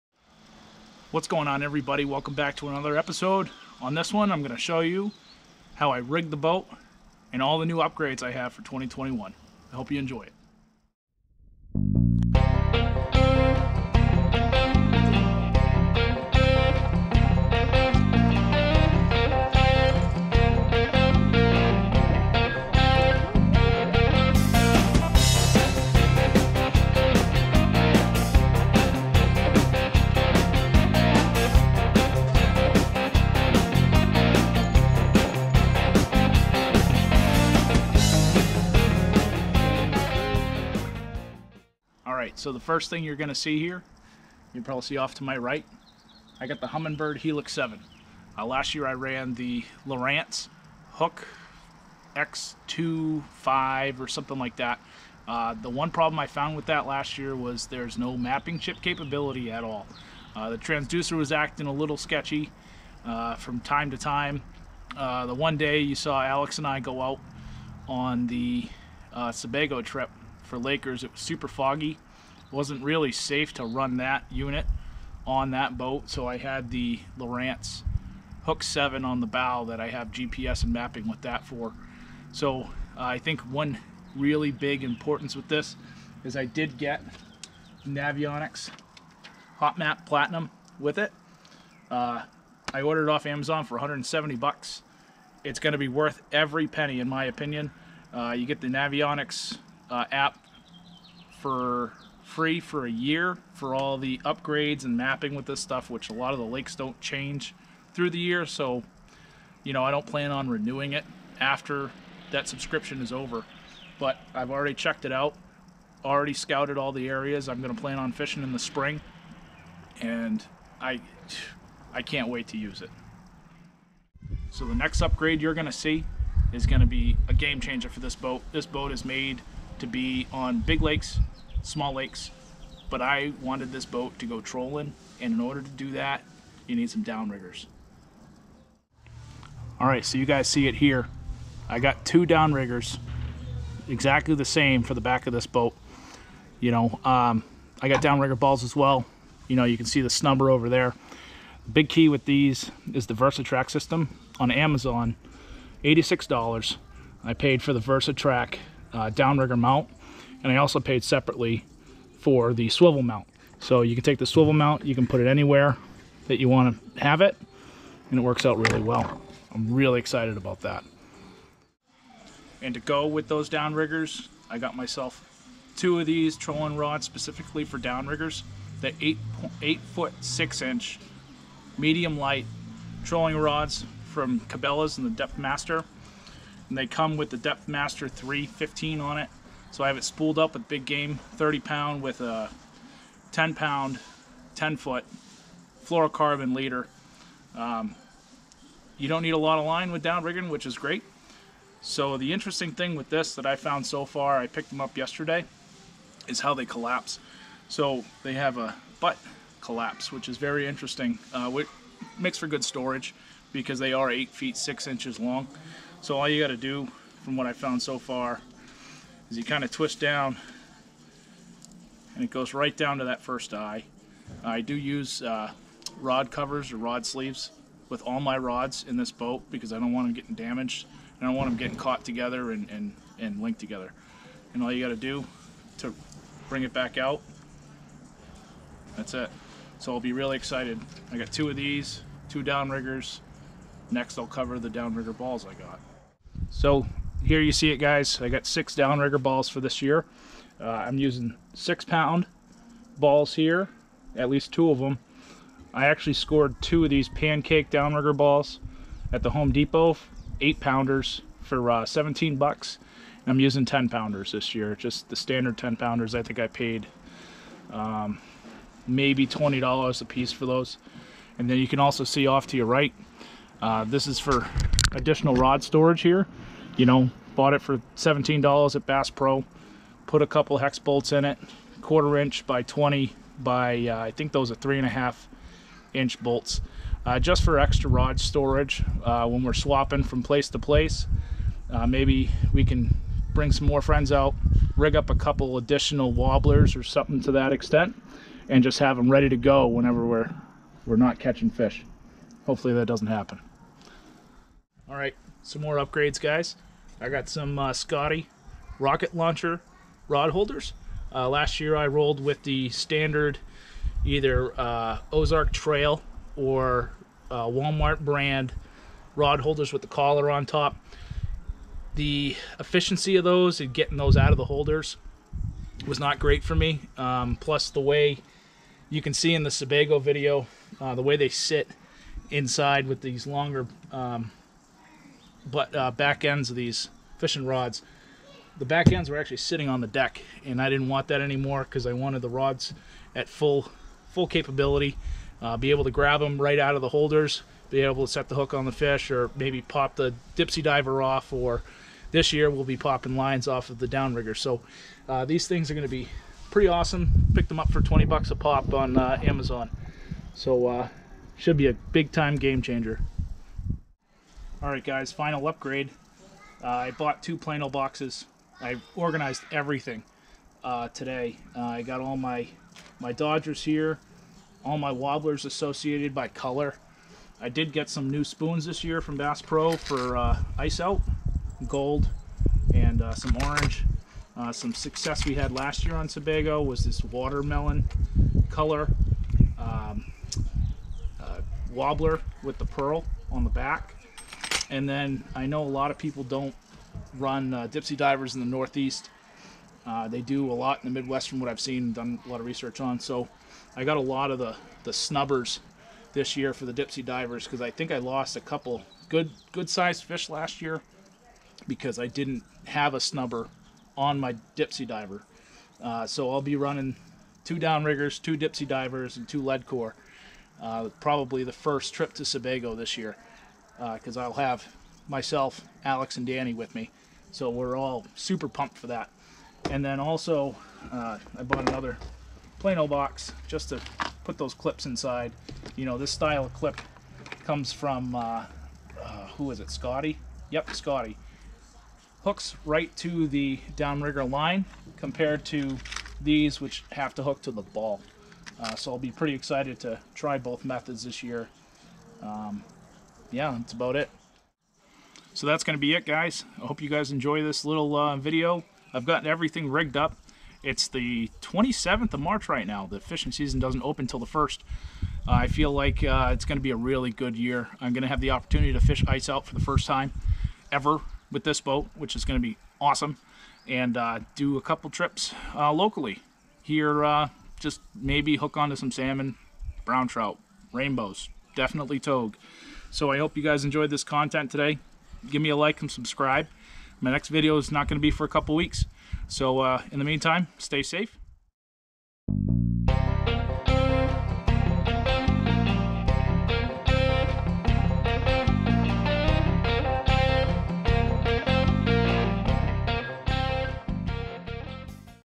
The Tracker Is Ready With MAJOR Upgrades!!!!
I added a Humminbird Helix 7 with the Navionics Hot Maps Platinum and some Cannon Downriggers.